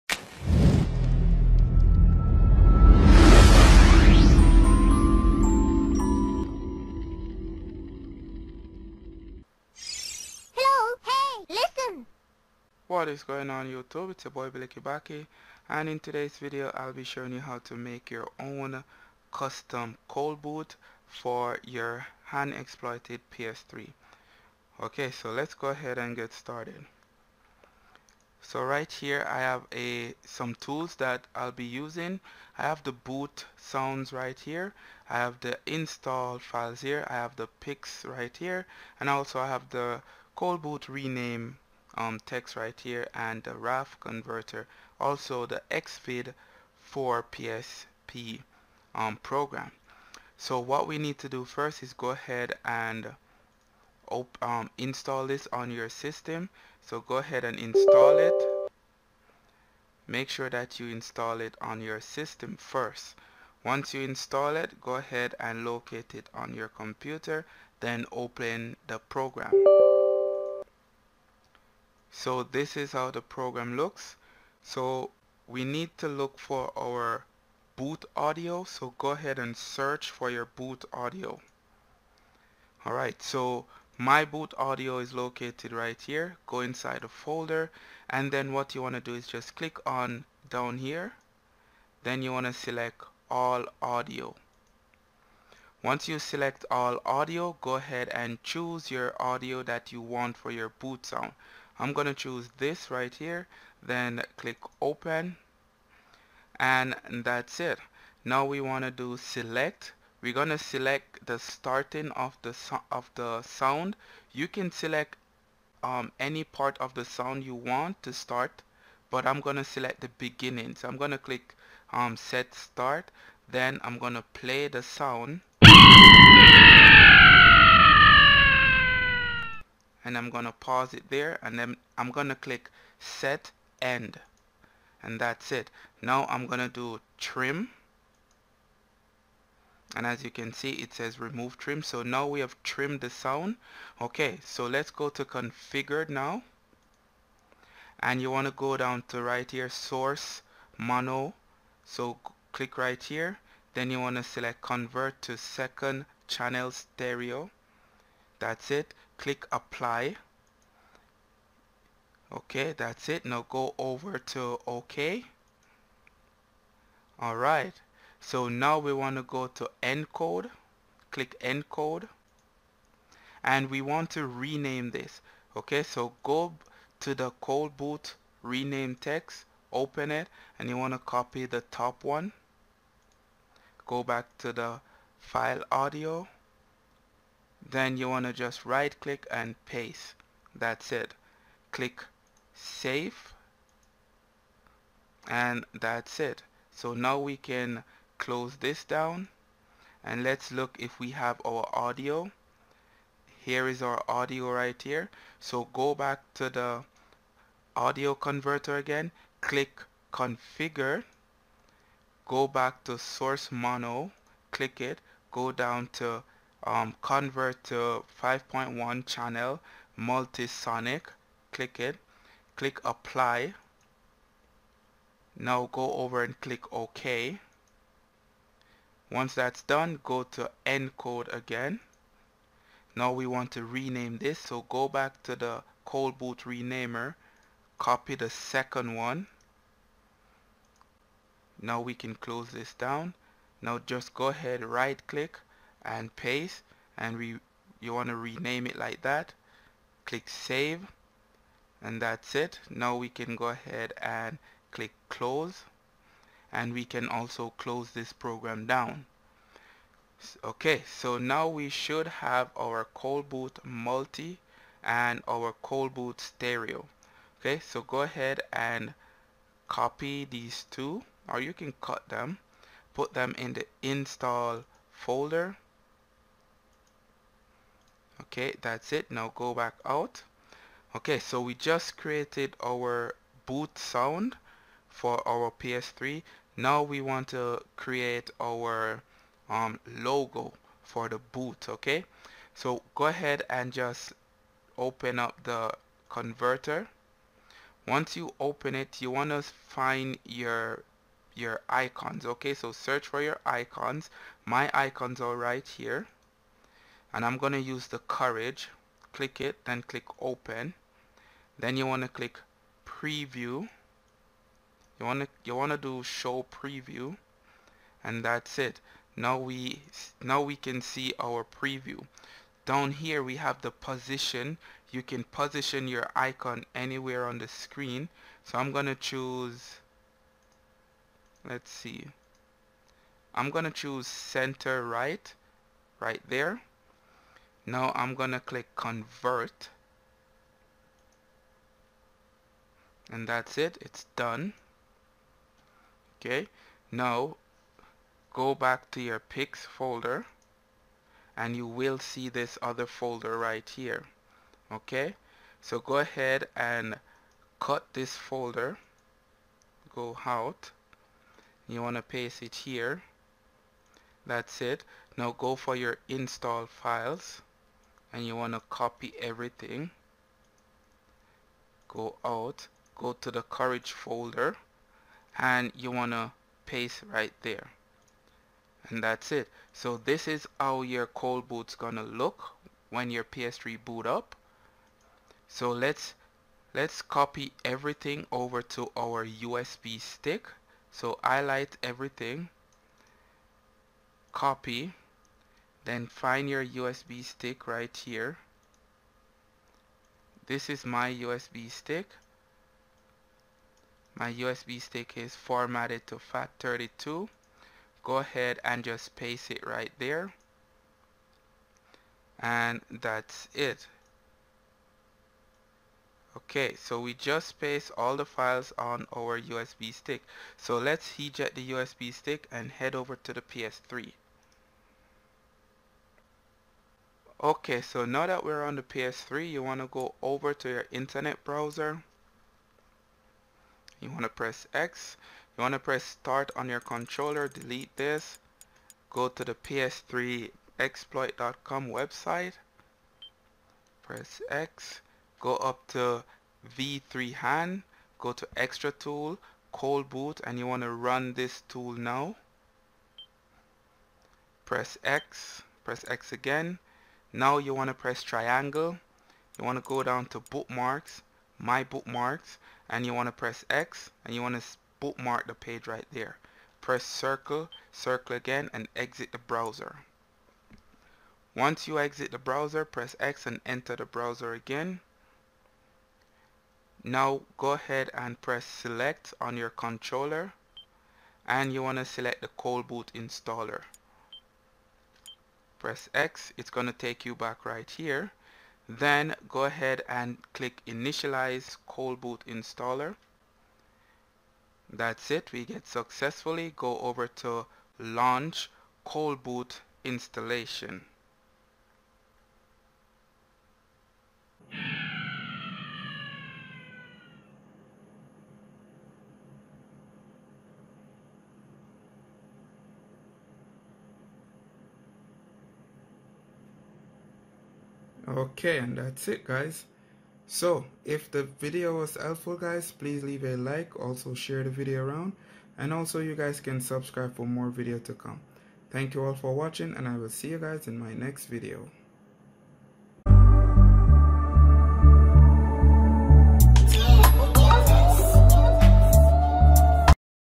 How To Make Your Own Custom ColdBoot With Boot Sound For HFW PS3 2020
Download